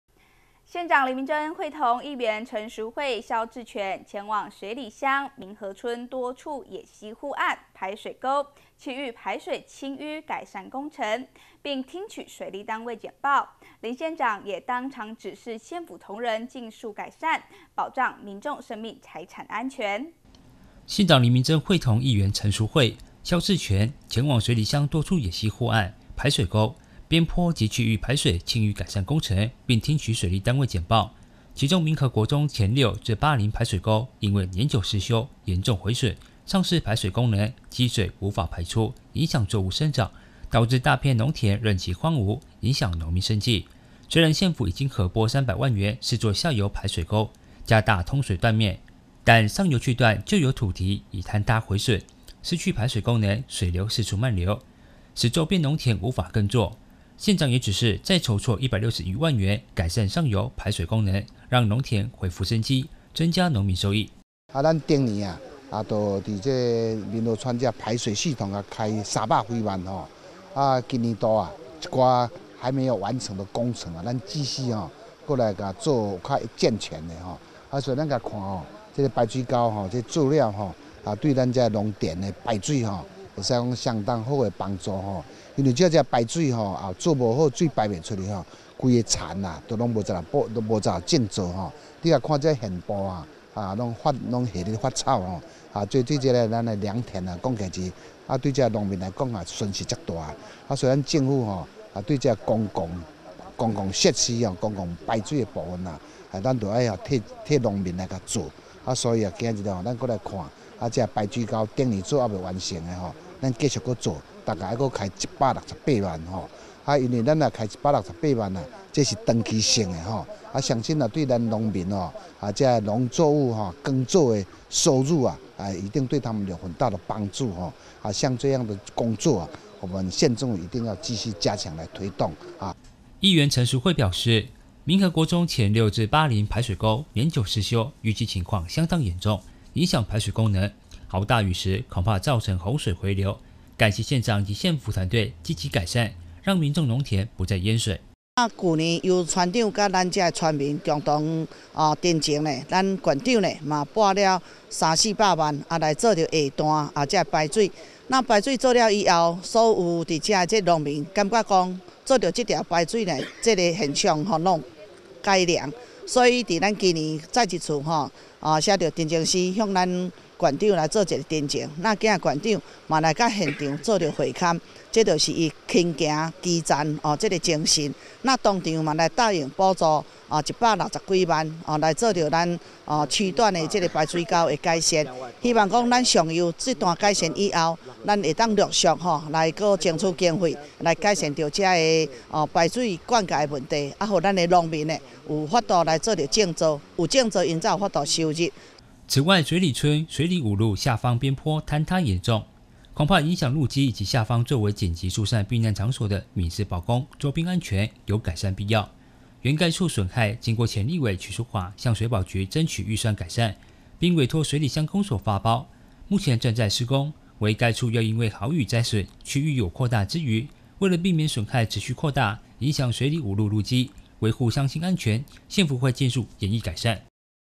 會勘水里野溪護岸排水溝 縣長指示儘速改善
縣長林明溱會同議員陳淑惠、蕭志全前往水里鄉民和村多處野溪護岸、排水溝、區域排水清淤改善工程，並聽取水利單位簡報。林縣長也當塲指示為避免損害擴大，危害居民生命財產安全，縣府會儘快改善，保障民眾生命財產安全。